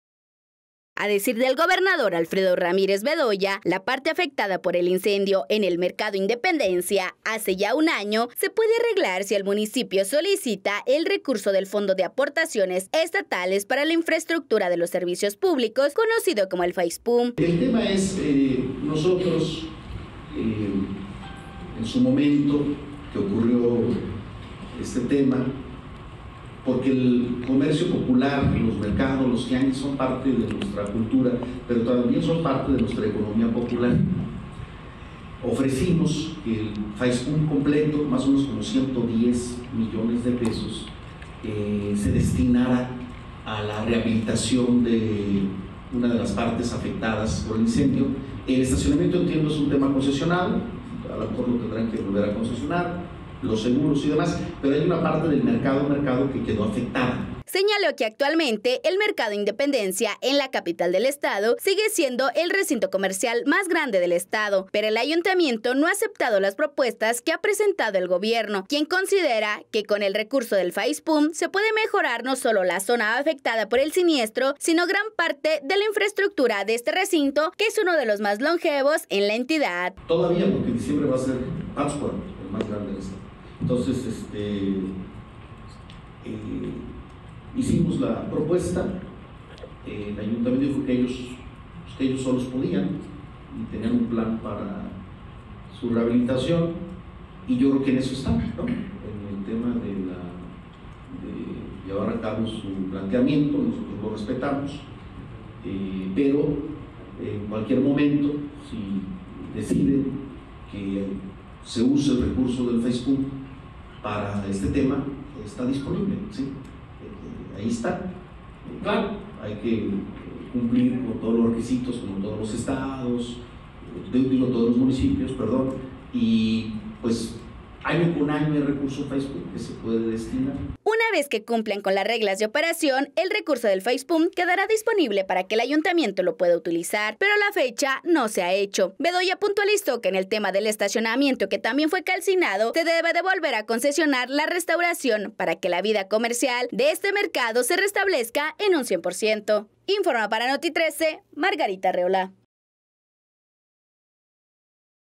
A un año del incendio del Mercado Independencia, aún no queda remodelado
Morelia | 🔴 En el marco del aniversario del incendio del Mercado Independencia, el Gobernador de Michoacán indicó que el recurso del FAIESPUM para el municipio, se puede destinar para remodelación de la parte afectada del complejo comercial.